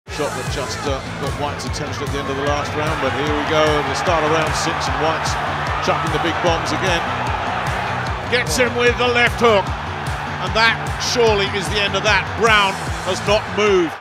Whyte BIG left hook stops Browne
Dillian Whyte floors Lucas Browne with a big left hook in their 2018 fight.